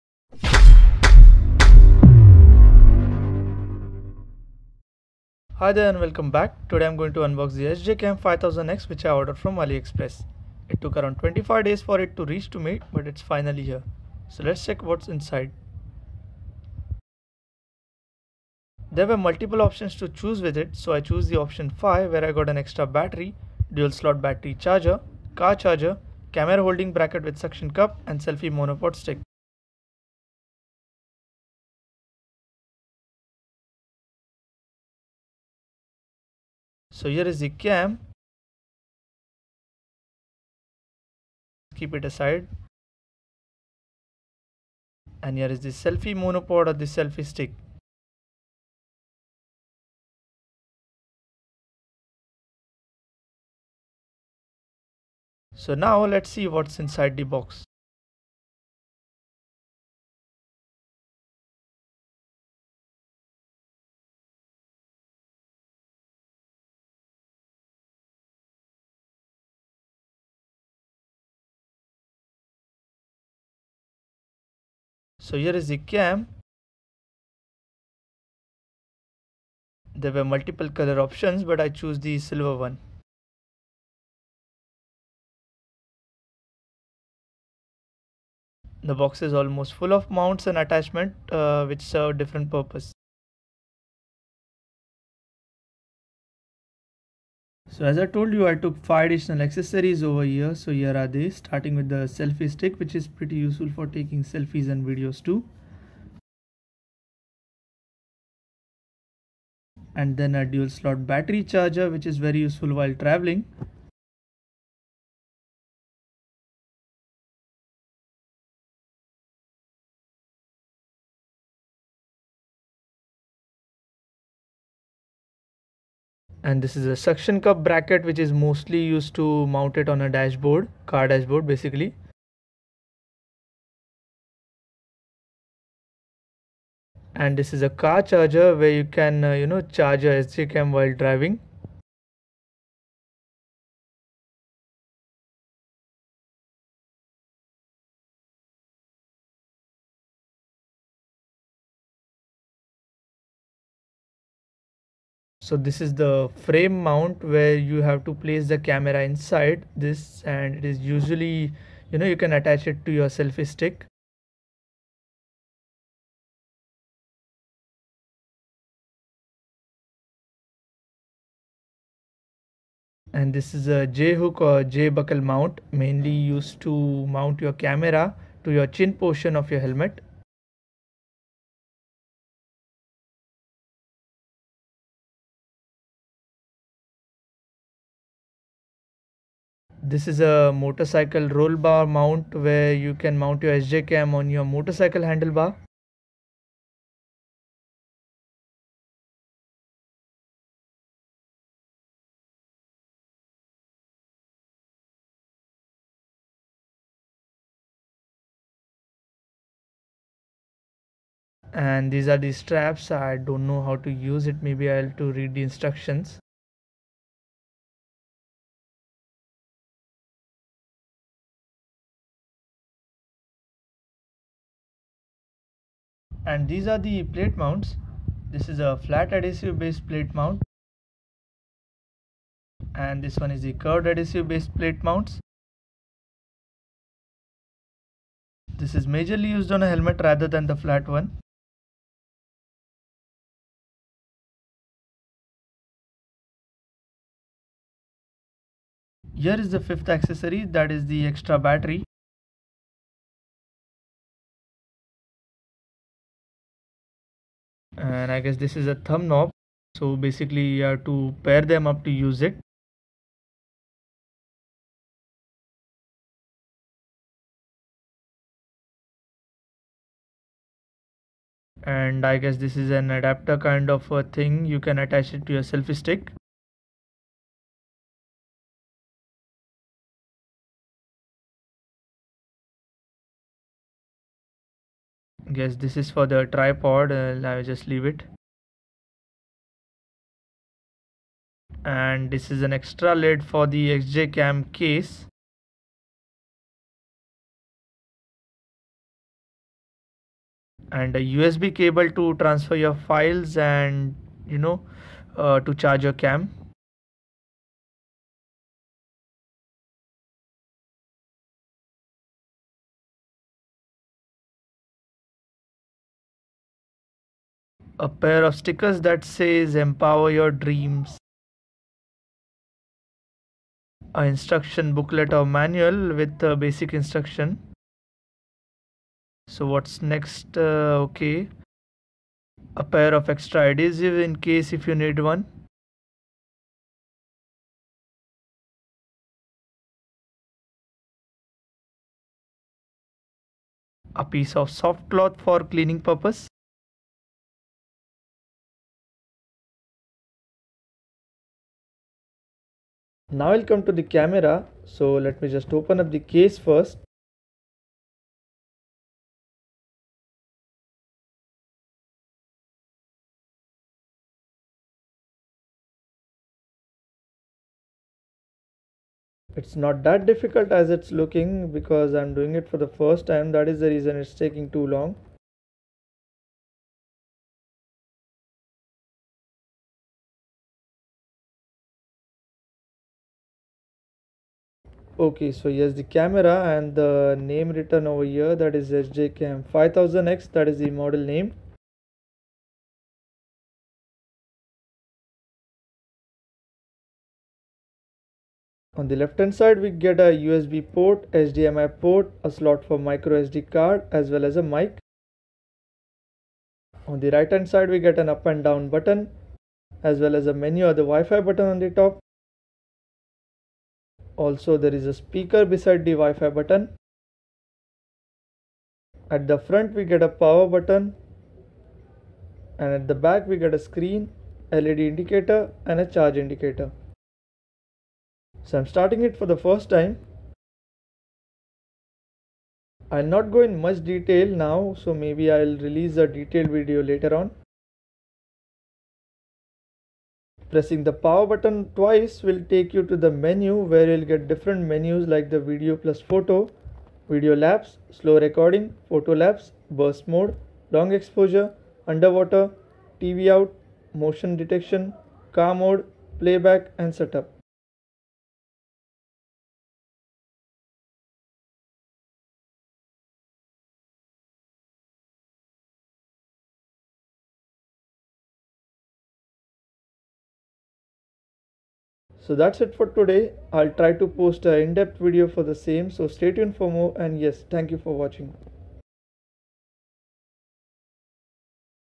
SJ Cam 5000x Unboxing (What's Inside)
A short unboxing about the SJ cam.
One of the best action camera in market